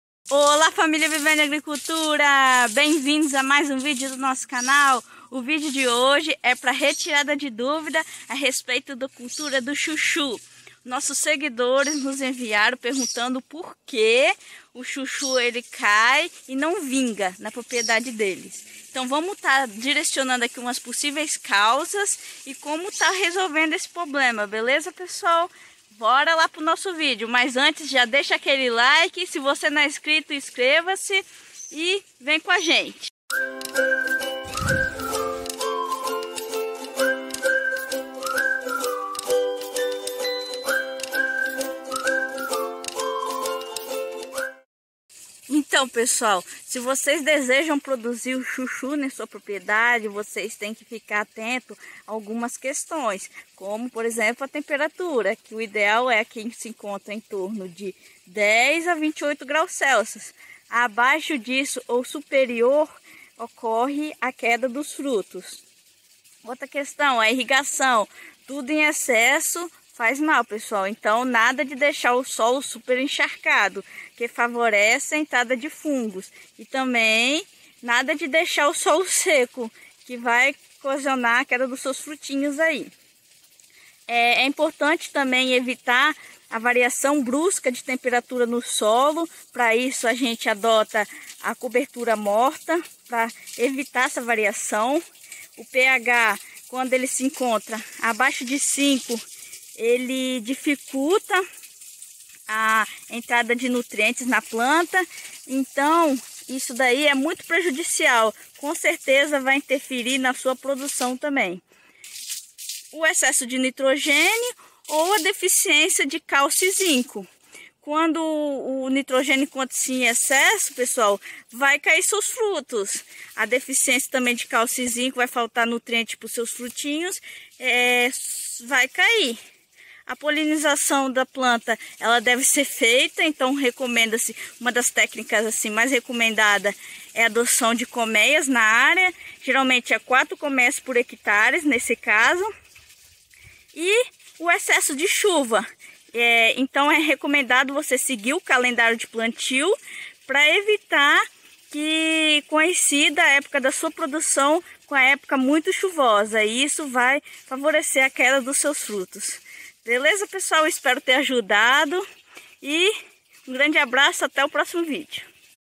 Seu pe de CHUCHU não da FRUTOS?? Entenda o MOTIVO e SOLUCIONE o PROBLEMA.
Os frutos do seu chuchuzeiro caiem antes mesmo de crescer??
Entre os fatores que podem provocar a queda precoce  do fruto de chuchu estão:
-Temperaturas abaixo de 10°C ou superior a 28°C logo, a solução é seguir o calendario para plantio de sua região, fazendo o plantio no momento certo.
- Irrigação ,  este processo tem que ser controlado de modo, a evitar o excesso de umidade que favorece a ploriferação de fungos parasitas de plantas.
-Variação brusca na temperatura do solo, para previnir que ocorra pode-se utilizar cobertura morta.
-PH abaixo de 5 e com presença de aluminio, a solução para evitar esse tipo de situação e fazer a analise do solo é a calagem, utilizando calcário para correção do PH nas dosagens corretas recomendadas por um profissional da area.
-Excesso de Nitrogenio ( este nutriente em excesso pode provocar o abortamento dos frutos logo, o nitrogenio deve ser aplicado de acordo com a necessidade mostrada na analise de solo.
-Deficiencia de calcio e zinco( o acrescimo correto destes nutrientes e baseado na analise de solo).
-Excesso de chuva pode derrubar os frutos logo, siga o calendario de plantio da sua região para que a epoca de produção não ocorra durante periodos chuvosos.
-Falta de insetos polinizadores ( adotar comeias em sua area pode ser a solução, lembrando que existem abelhas de ferrao e as sem ferrão e o recomendado é de duas a quatro comeias por hectare.